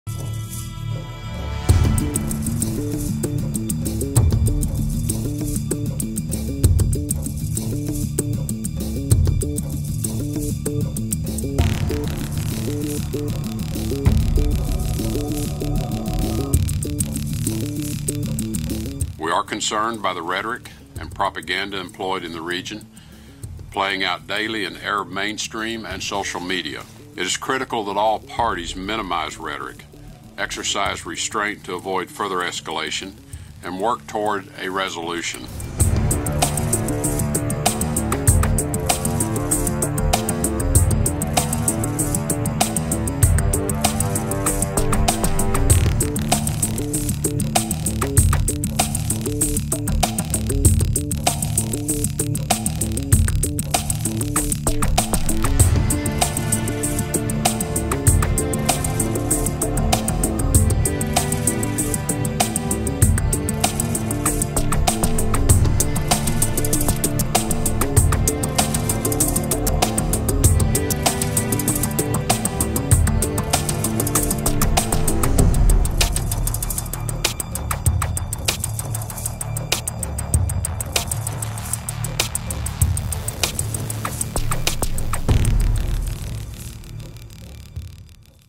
حوار استراتيجي بين قطر والولايات المتحدة
حوار استراتيجي بين قطر والولايات المتحدة انتهى بتوقيع مسؤولي الدولتين اتفاقات أمنية واقتصادية وتكنولوجية، بعد ثمانية أشهر من الأزمة الخليجية.